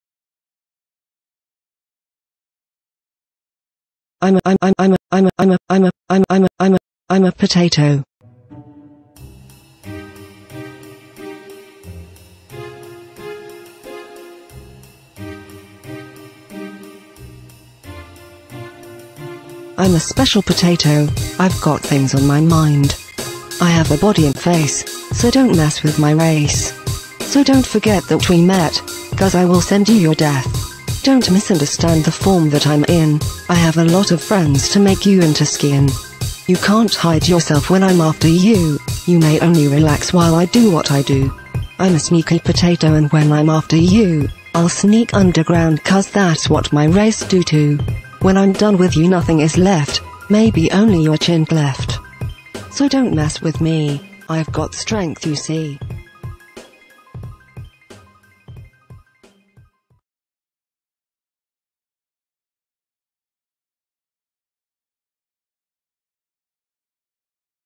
The badass potato
A potato rap's about how badass he is.